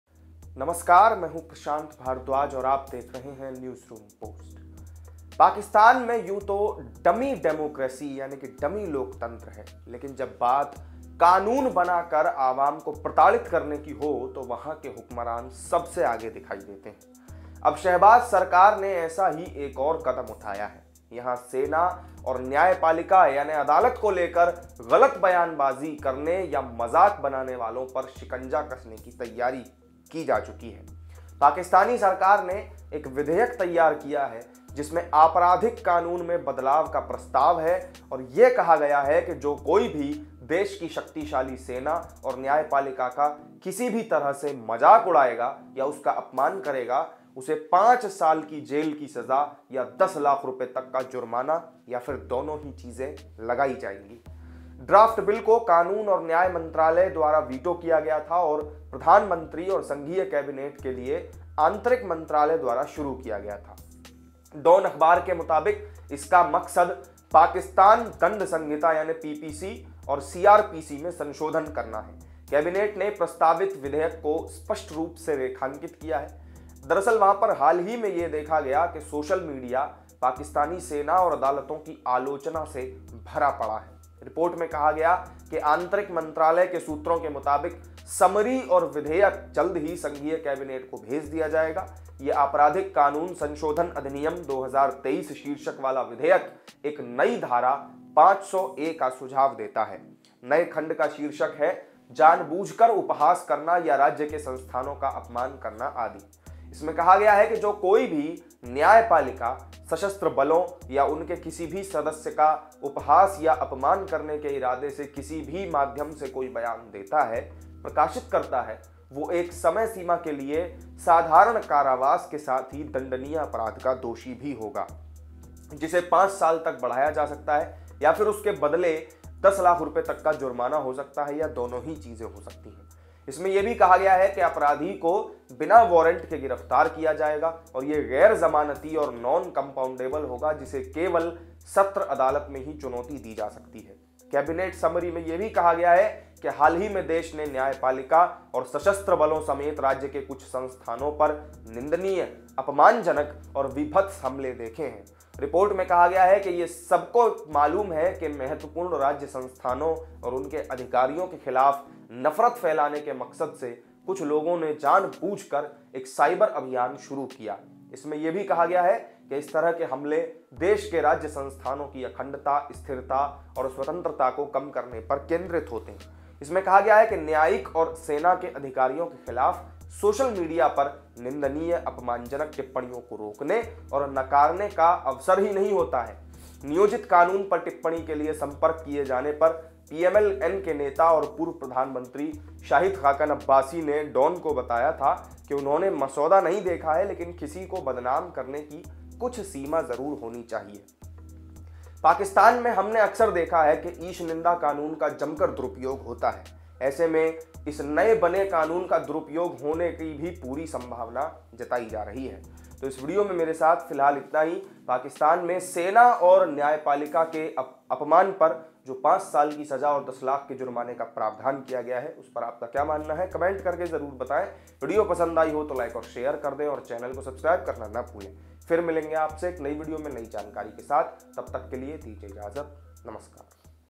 Pakistan New Bill | पाकिस्तान में सेना और कोर्ट के खिलाफ बोलने पर होगी 5 साल की जेल..पेश हुआ विधेयक
Pakistan New Bill | There is a dummy democracy in Pakistan...But when it comes to making laws and harassing the people, then the rulers there are seen at the forefront...Now the Shahbaz government has taken another such step...Here the army and There is a preparation to crack down on those who make false statements or jokes about the judiciary... The Pakistani government has prepared a bill which proposes changes in the criminal law and says that anyone who uses any part of the country's powerful army and judiciary ridicules or insults by any means, shall be punished with imprisonment of either description for a term which may extend to five years, or with fine which may extend to ten lakh rupees, or with both.